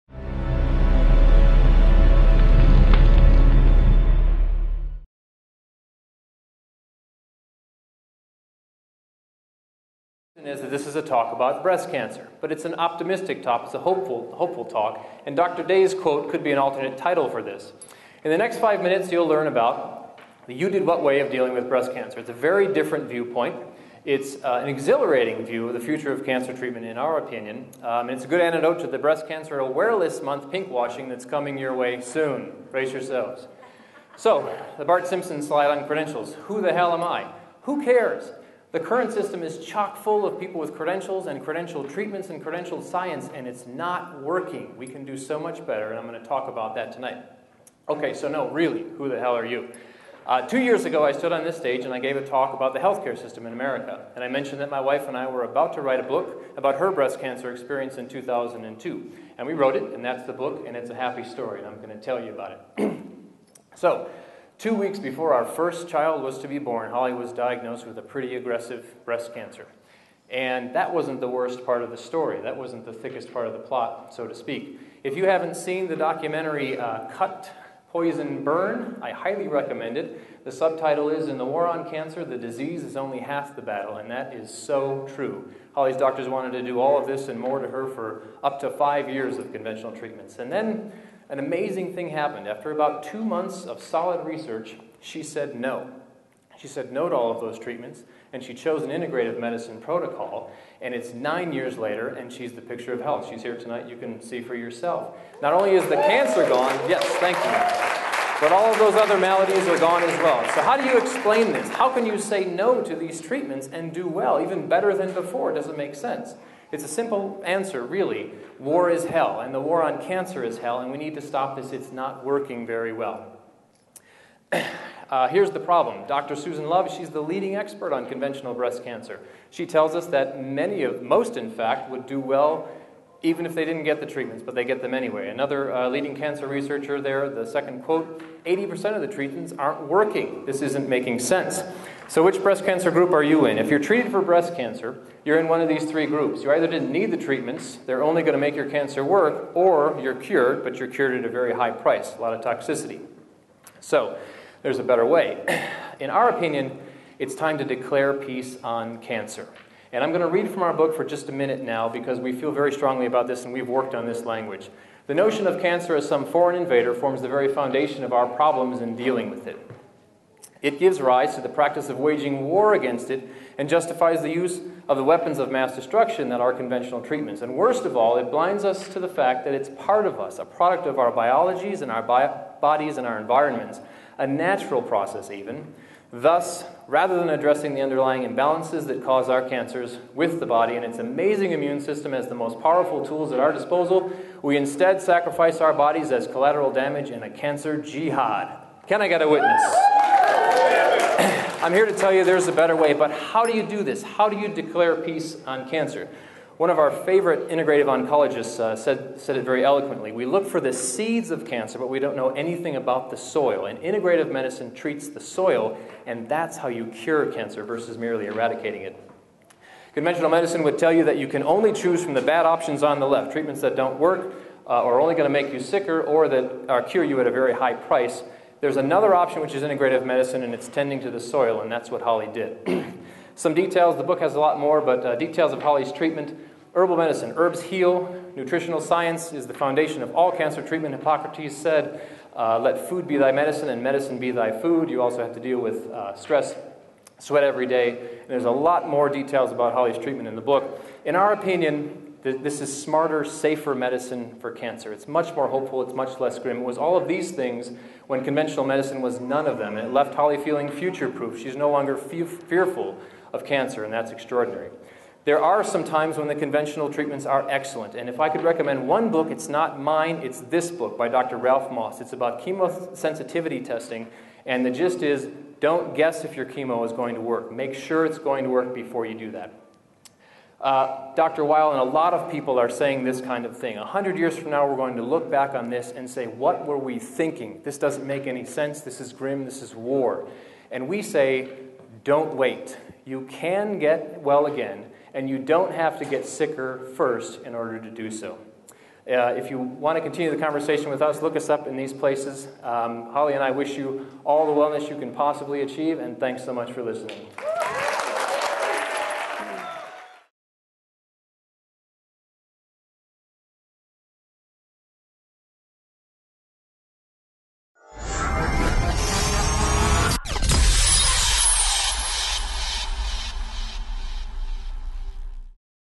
Ignite Fort Collins 9: Patrick Quinn - "Ladies, I love your Breasts (I'll Explain!)"
"Ladies, I love your Breasts (I'll Explain!)" - Presentation given on September 8th, 2011 at Ignite Fort Collins 9 by Patrick Quinn.